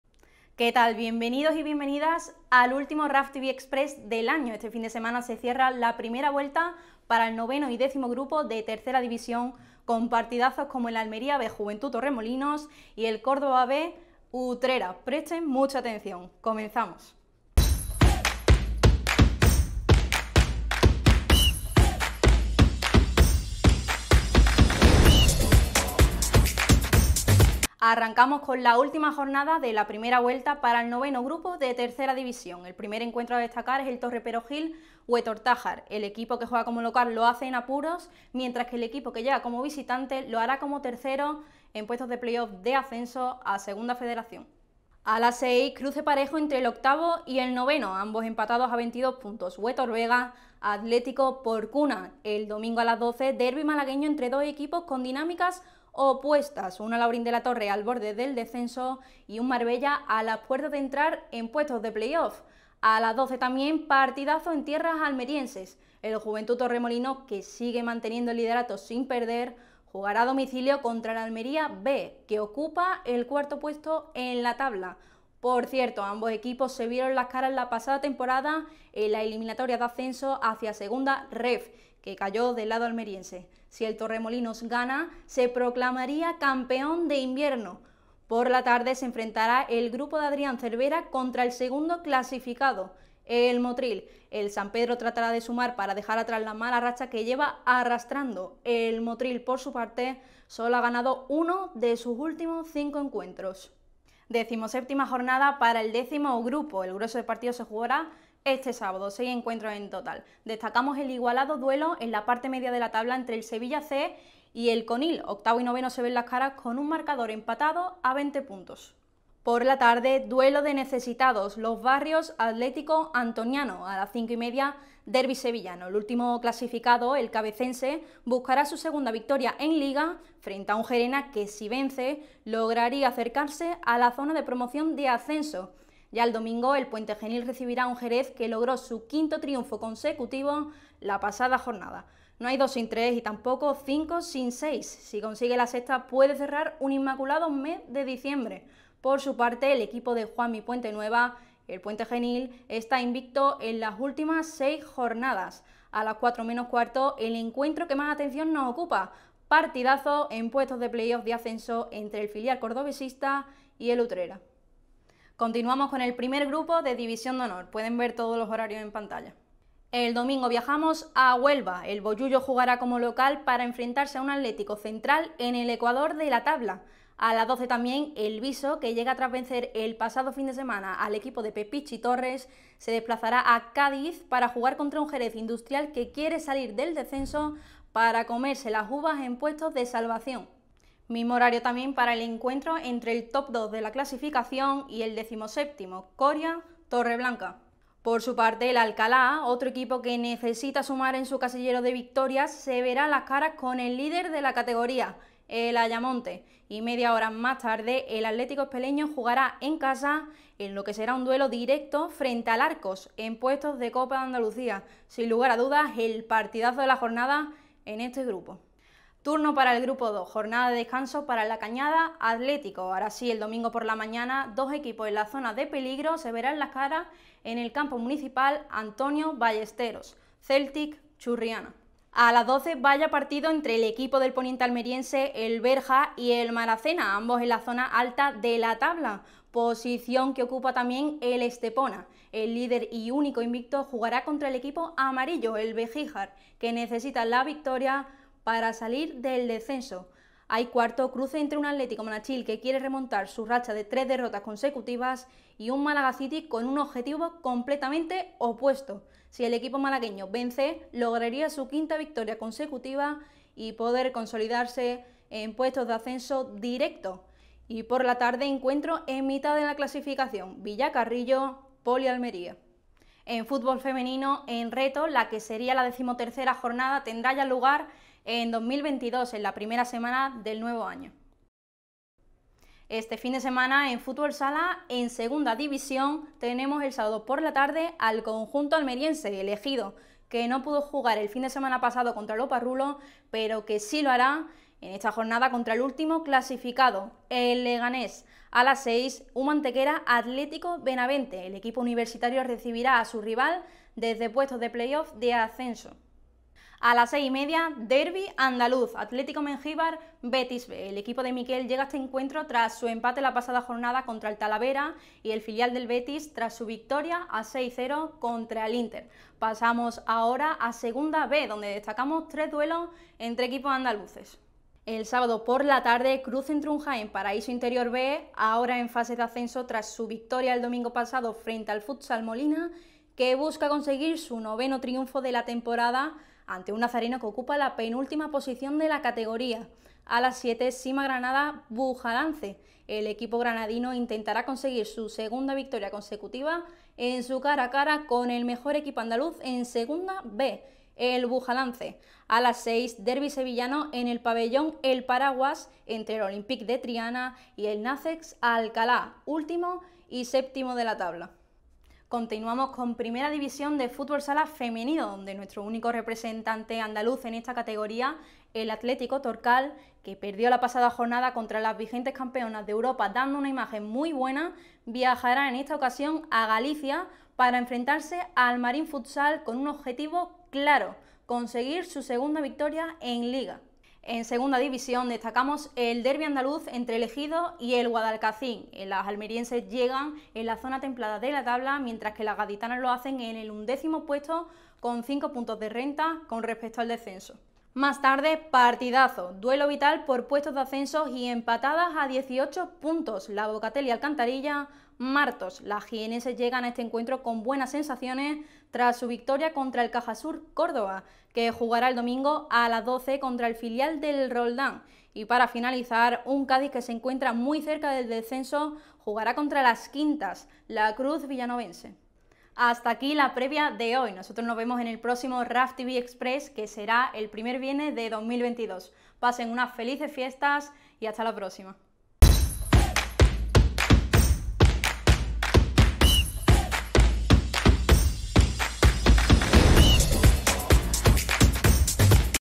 RFAFtvExpress 1x08
Nueva entrega de RFAFtv Express, con lo mejor que nos deparará el fútbol andaluz en las jornadas del 18 y 19 de diciembre.

Conoce la previa de la jornada para los equipos andaluces en TerceraRFEF DHS Futsal y Futsalfem